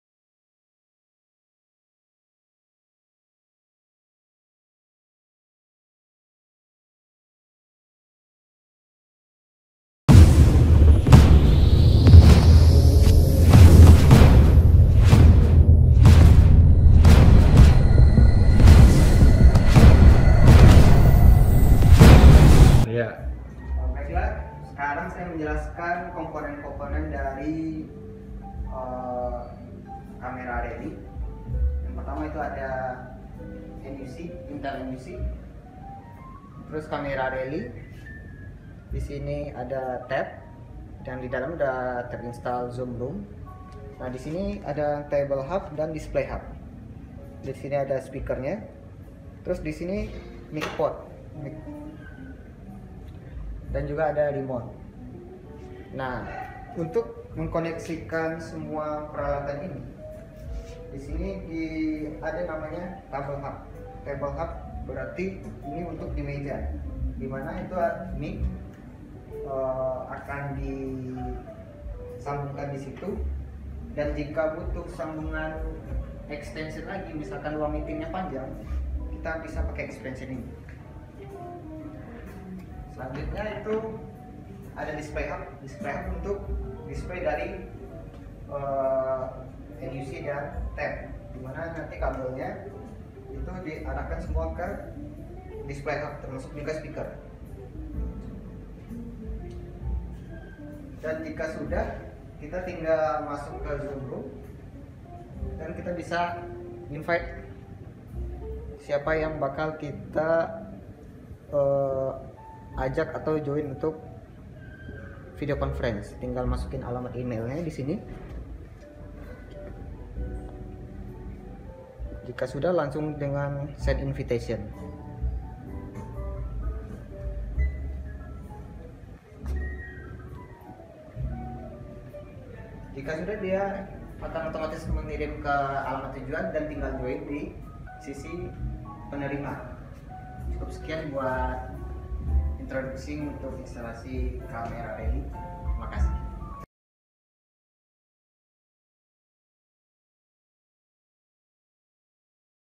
Logitech Rally Components & Interconnection
Explain Logitech Rally components and how it connected each other.